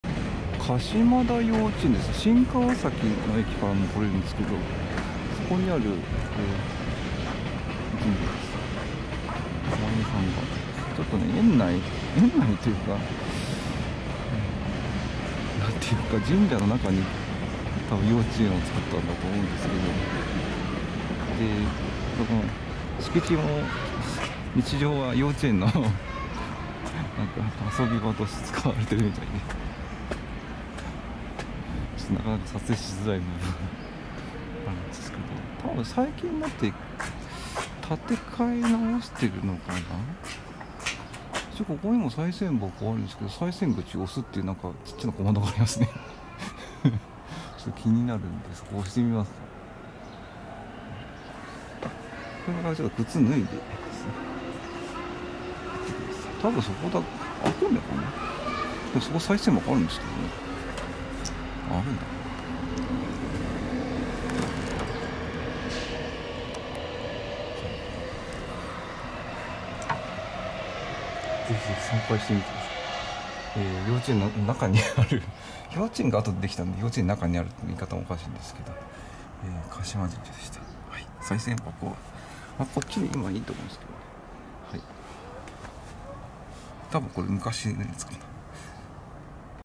鹿島大神 鹿島田幼稚園内 多分土日なら参拝は良いんだと思う
幼稚園の中といえば良いのか　神社の宮司様が幼稚園をやっているのかもしれないいわゆる境内の敷地は幼稚園のグラウンド的な
土日参拝なら問題ないと思います

20～30万以上神社があります。　実はコンビ二よりも多い